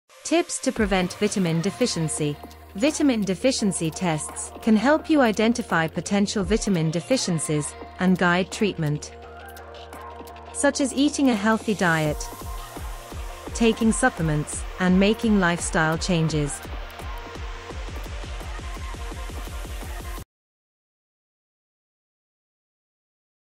tips to prevent vitamin deficiency
vitamin b12 deficiency vitamin b12 deficiency symptoms b12 deficiency symptoms

Feeling sluggish or experiencing frequent illnesses? It might be time to check for vitamin deficiencies! Our engaging video is filled with practical advice on preventing nutrient gaps in your diet. From colorful recipe ideas packed with vitamins to mindful habits that support optimal health, we've got it all covered! Take the first step towards a healthier life - don't wait any longer, watch our enlightening video now!